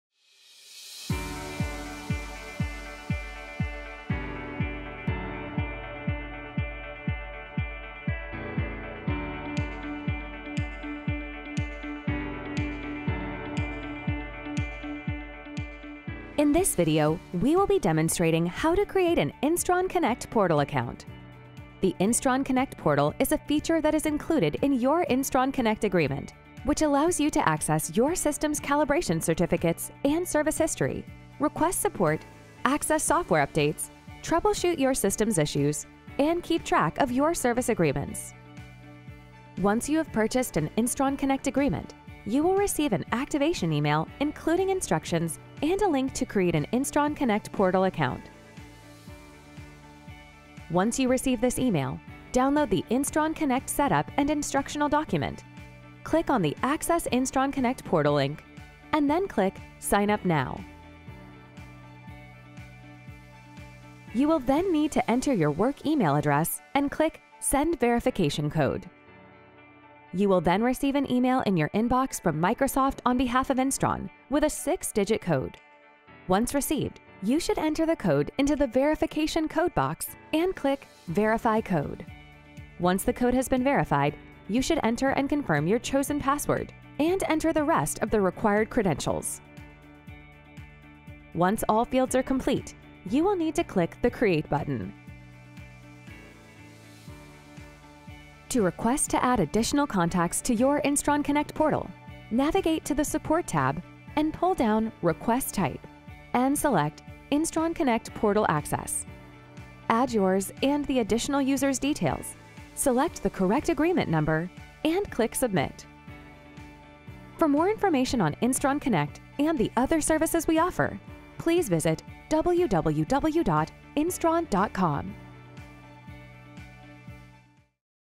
Instron® Professional Services | Instron Connect Portal | Creating an Account
The Instron Connect Portal is a feature that is included in your Instron Connect agreement, which allows you to:
- Access your system’s calibration certificates and service history
- Access your service agreement
- Request support
- Download software updates
- Troubleshoot your system’s issues